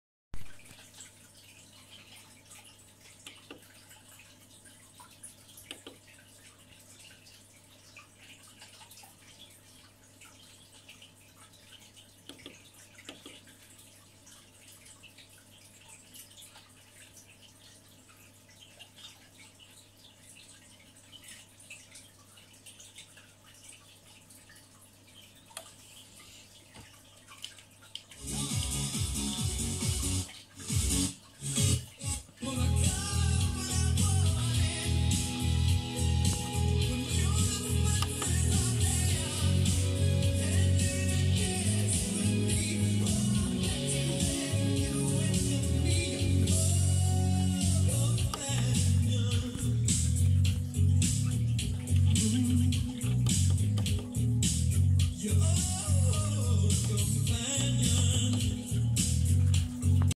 Radio PLUS na 96,5 FM z RTCN Warszawa *Pałac Kultury i Nauki* na SONY ST-390 - 21.01.2021
Stały odbiór, z 9 Elementowej Yagi Dipola ustawionej pionowo 117 KM od nadajnika.